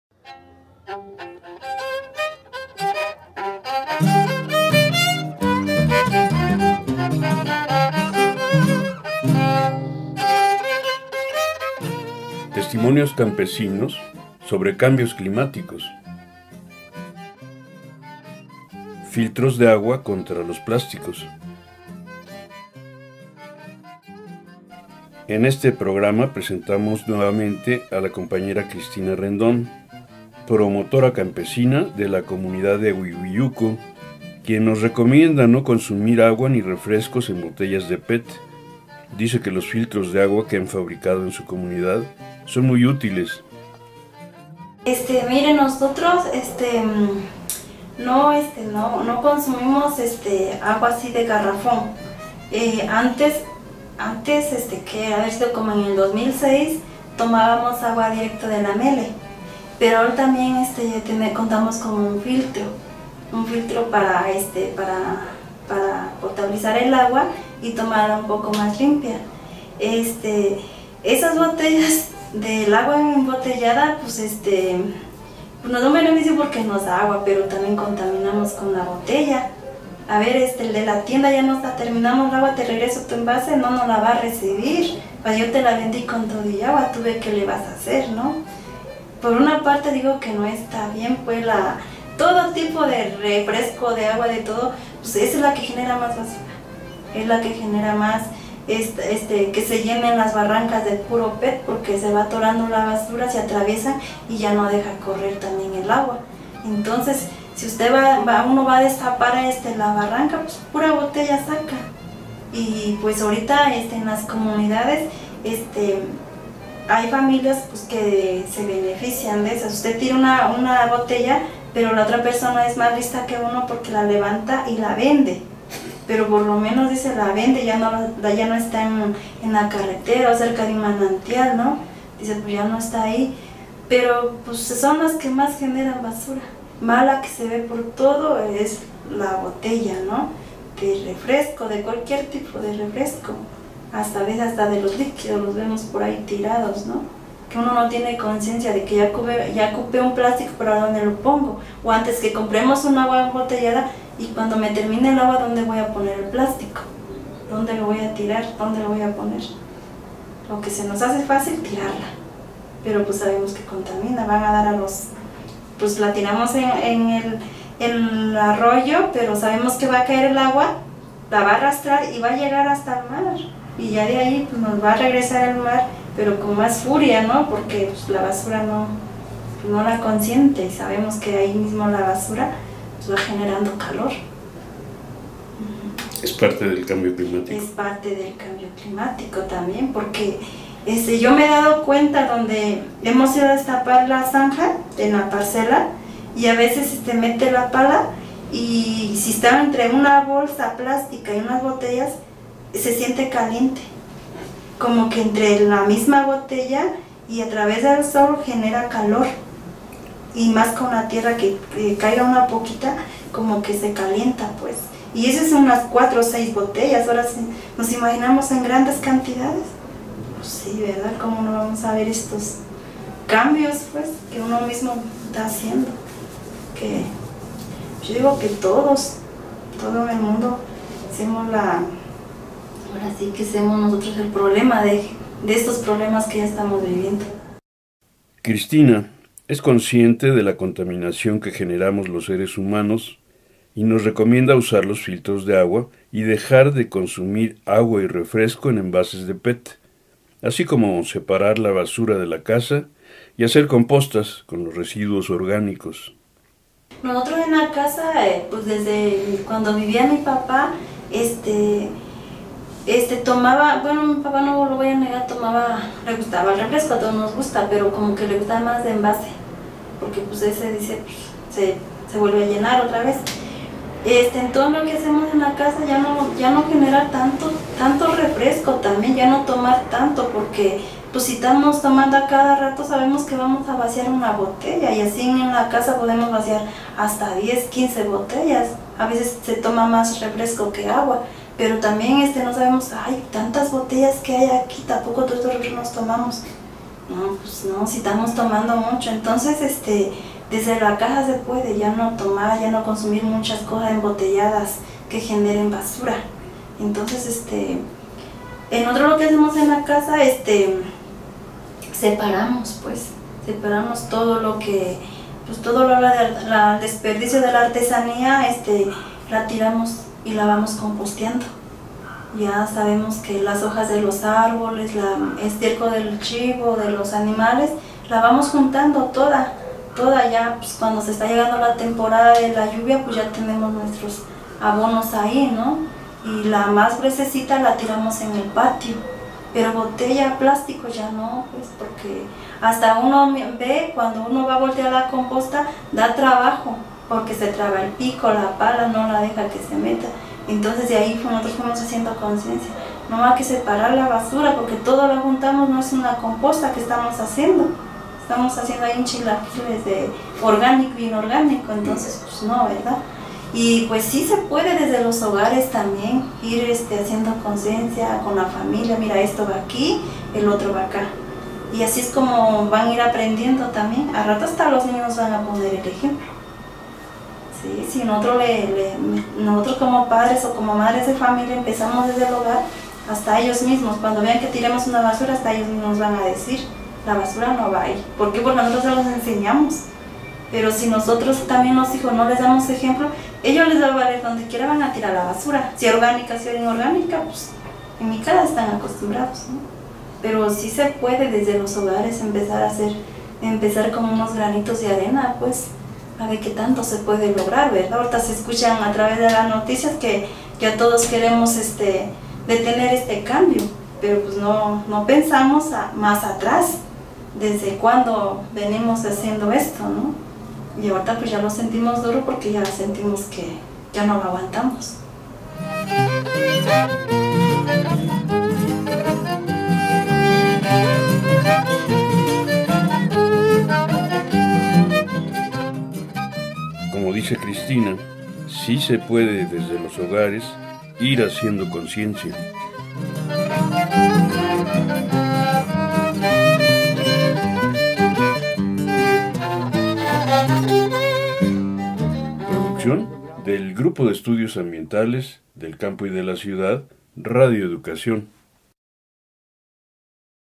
13. Filtros contra plásticos / Testimonios de promotoras y promotores campesinos de Guerrero
Segunda parte:
Testimonios campesinos sobre los cambios climáticos

1. Testimonios de Guerrero

Testimonios de promotoras y promotores campesinos de Guerrero

13. Filtros contra plásticos
(Cristina Rendón, Ahuihuiyuco)

1. Lecturas sobre el cambio climático

2. Testimonios campesinos sobre los cambios climáticos

Créditos:

Producción
GEAVIDEO

Catherine Marielle

Mexico-Tenochtitlan 2019.